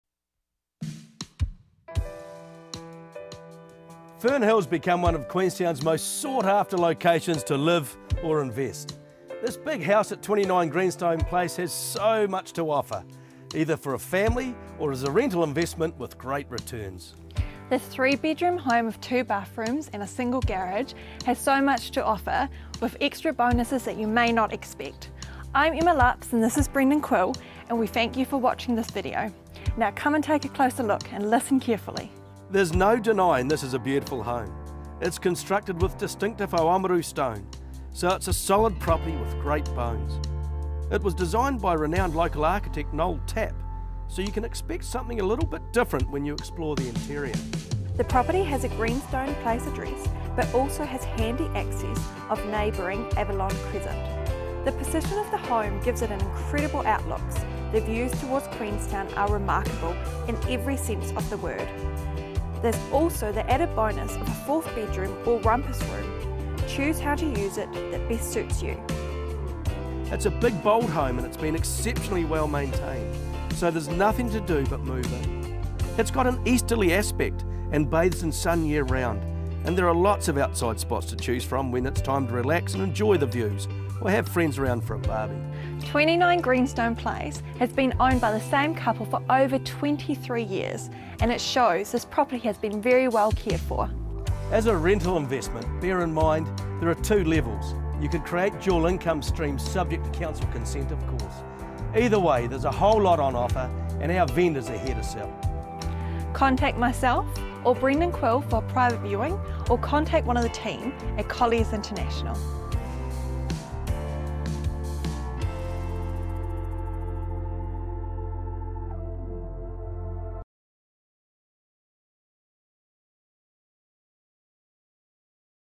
PressPLAY Property: 29 Greenstone Place, Fernhill, Queenstown
Big, bold and beautiful
In one of Queenstown's most sought-after locations to live and invest, this big, bold house opens up so many opportunities as either a family home or rental investment with great returns.

The beautiful home was constructed with distinctive Oamaru stone and designed by renowned local architect Noel Tapp, with many added extras throughout. Owned by the same couple for 23 years, it's been exceptionally well-maintained, so there's nothing to do but move in and make it your own.

In addition to the three bedrooms there's a large rumpus room, or fourth bedroom. The separate laundry room with outdoor access adds convenience when returning from Queenstown's popular outdoor activities, whilst a bathroom on each level offers space for everyone. Explore your options - with two bedrooms on each floor, there could be the possibility to separate out the two levels as dual income streams, subject to council consent.

The home has easterly aspects, year-round sun, epic Queenstown views and plenty of outside spots to enjoy. With so many options inside and out, why not relax with a good book, soak up the stunning surrounds with a favourite beverage, or invite friends and family round and heat up the barbeque. Truly a home for all seasons, view today.

• Big, bold three bedroom, 2.5 bathroom family home with rumpus room
• Remarkable Queenstown views and year-round sun
• Outdoor living areas include BBQ, balcony, pergola and deck
• Huge rental opportunity potential
• Dual access off Greenstone Place and Avalon Crescent

MB01E0UV0ASMORJ